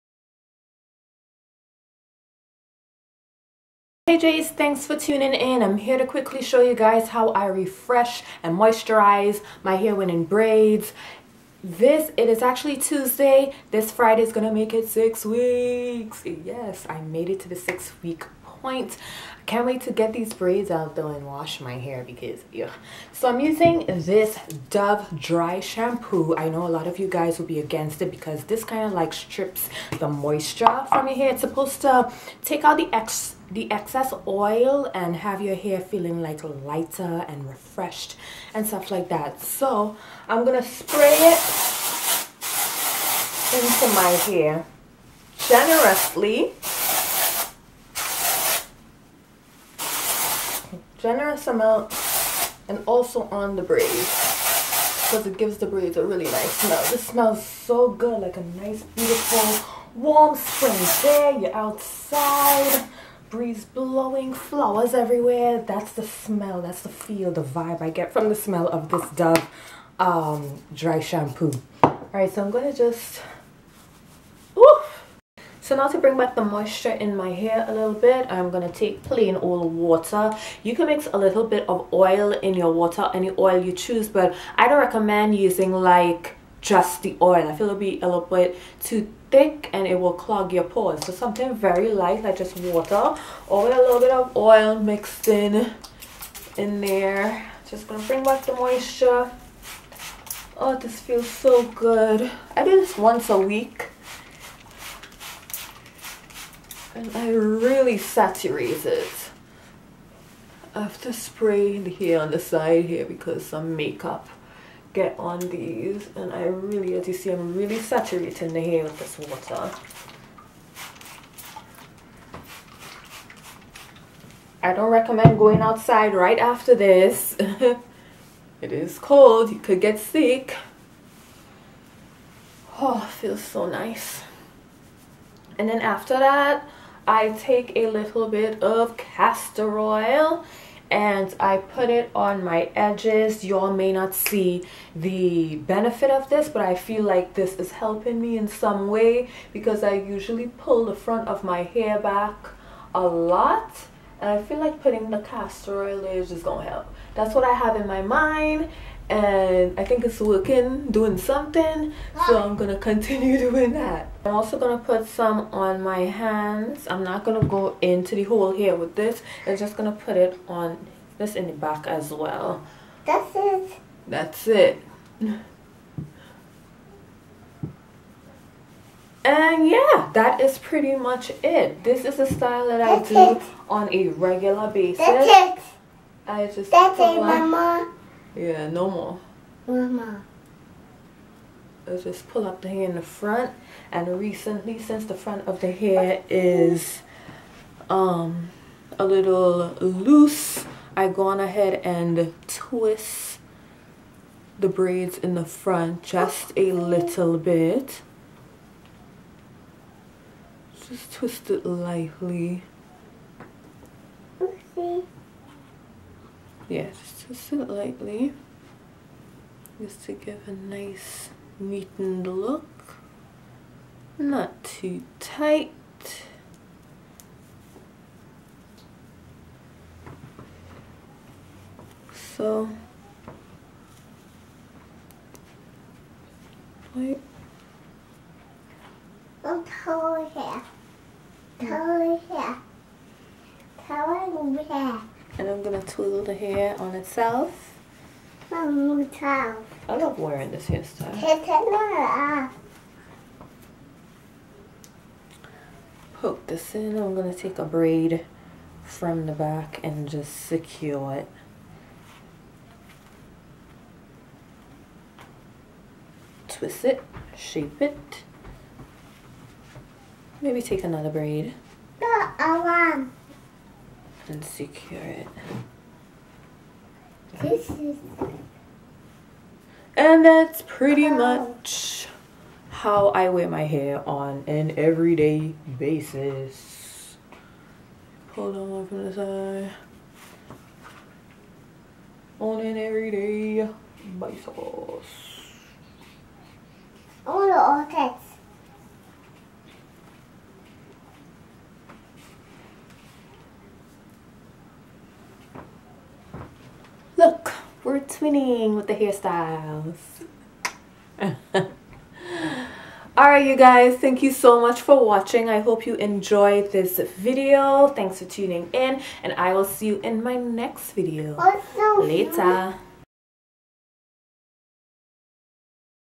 REFRESH + MOISTURIZE Hair when Braided
This is what I do on a weekly basis

Note- for the first two weeks I did not use dry shampoo

Blessings
J